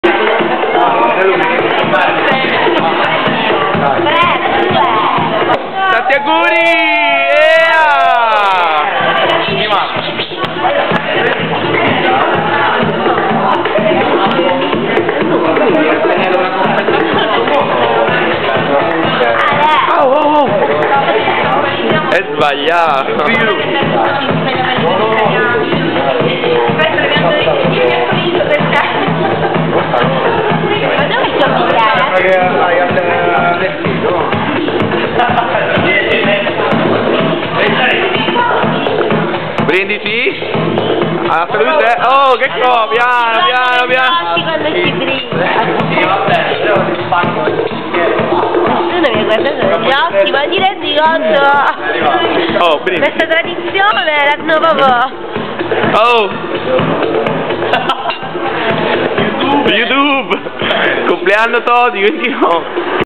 Compleanno Toti 29 anni
Al AvidaDiva con gli amici si festeggia il 29esimo compleanno di Toti dj